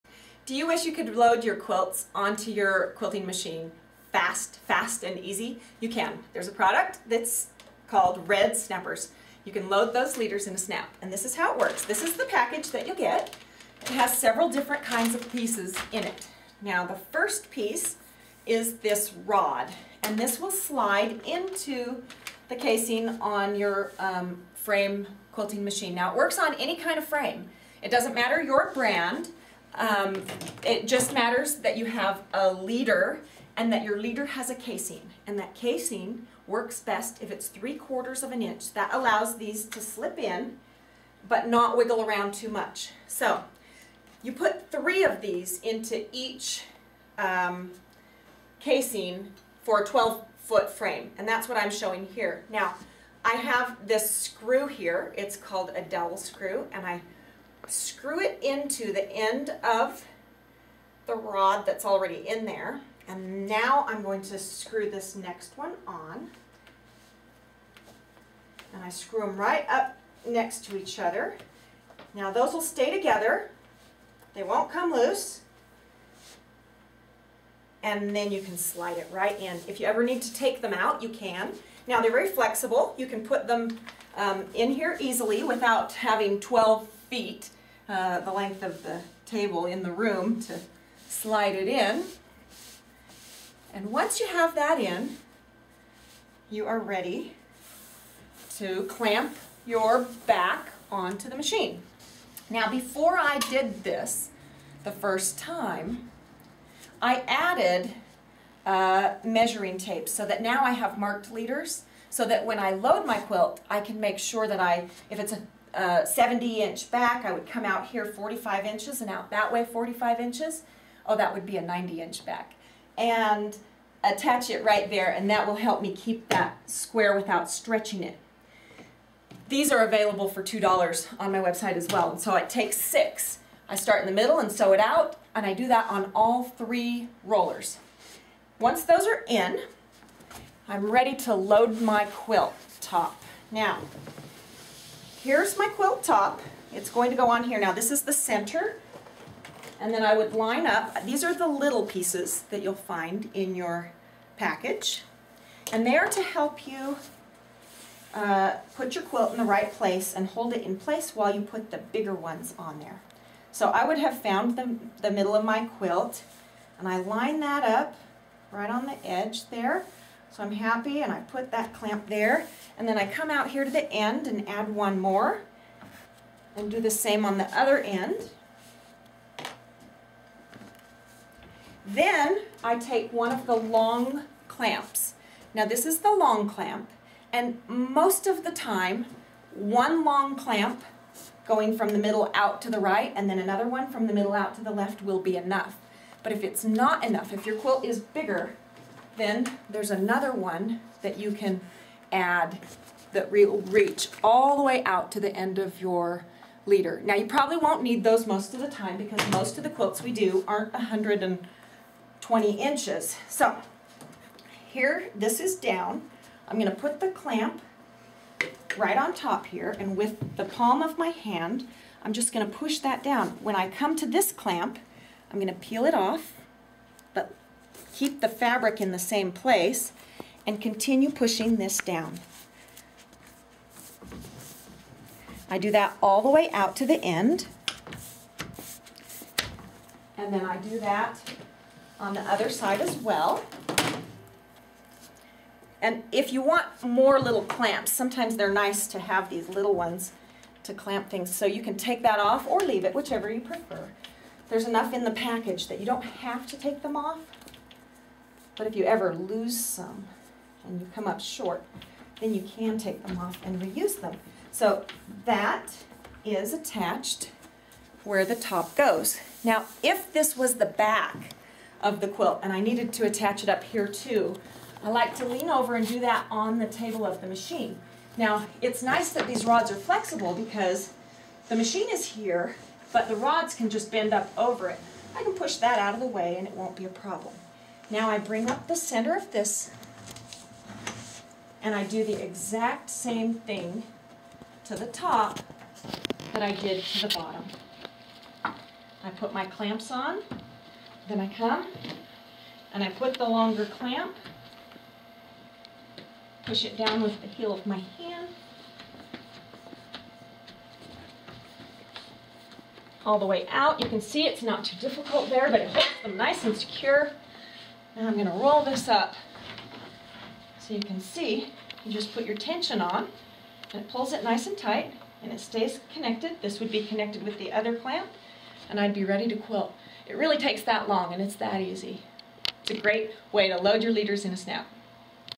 red snappers.MOD
Renae's Red Snappers are a great product, made by clip-n-seal, designed to load quilts on quilting frames.  Shown here on the Innova, but they work with any brand frame.  They eliminate the need for pins, staples, Velcro and zippers.  No more pins to snag your clothes or poke your fingers!  Available in various lengths for different frames